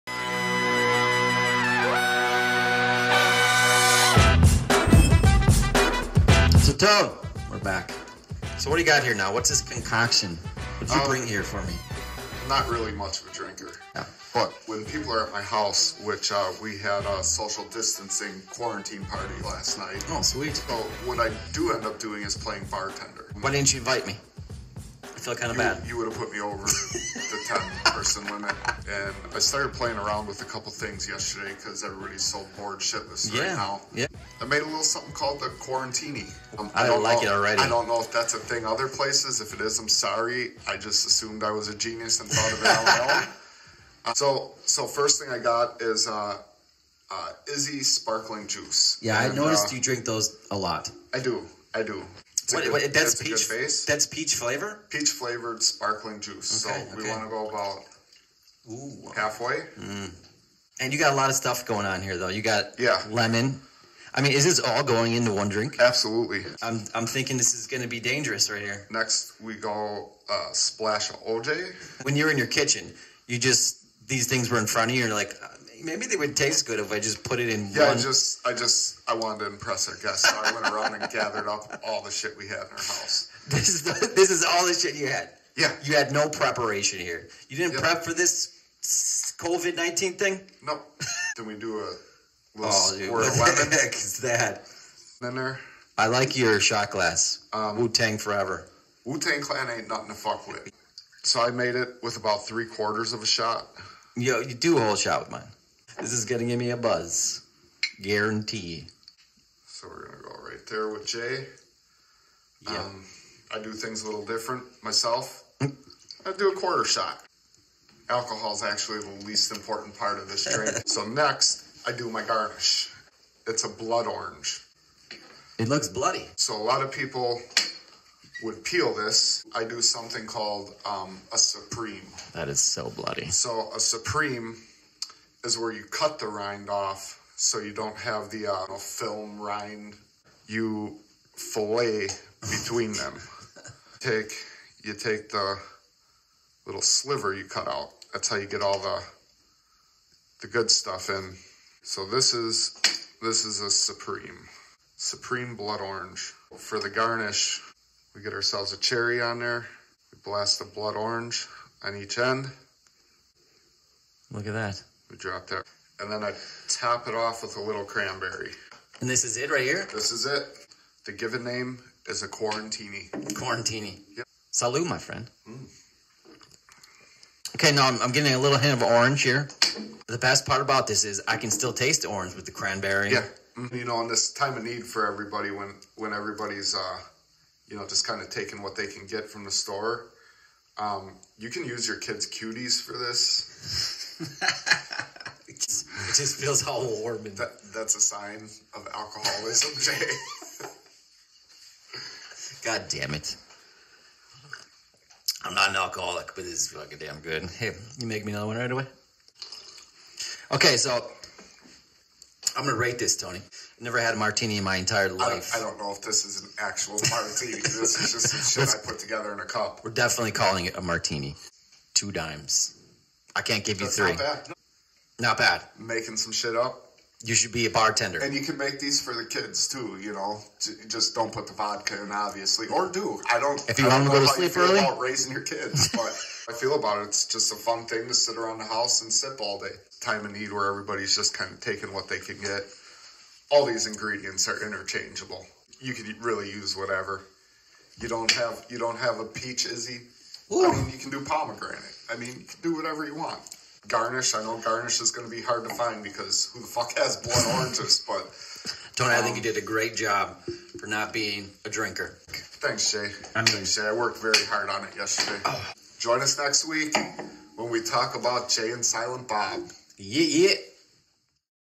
Quarantini how to make
Being quarantined during the covid-19 pandemic we dabble around with mixers to make a martini.